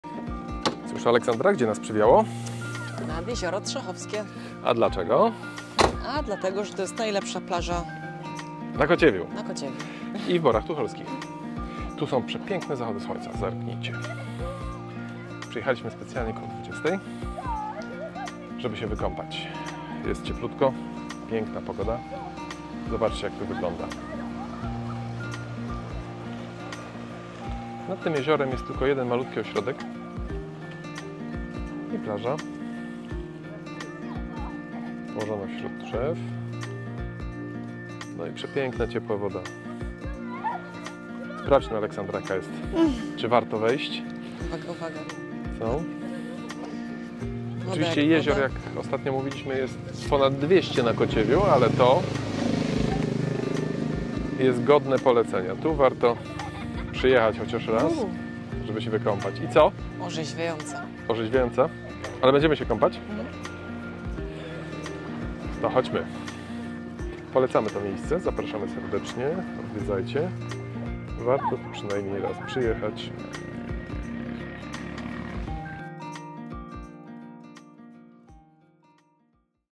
Piękna plaża nad jeziorem Trzechowskim
Dziś zabieramy Was nad malownicze Jezioro Trzechowskie. Oczywiście położone na Kociewiu. 🇵🇱
Przyjechaliśmy tu wieczorem, żeby uchwycić magię zachodu słońca nad taflą wody.  Plaża jest tutaj urokliwa, z szerokim i płytkim zejściem do wody, idealnym na letnie kąpiele. ️
W oddali widać majestatyczną wieżę kościoła w Piecach, która dodaje temu miejscu jeszcze więcej uroku. ⛪️
Jeżeli szukacie pomysłu na spokojny wieczór w pięknej scenerii, to koniecznie obejrzyjcie nasz film!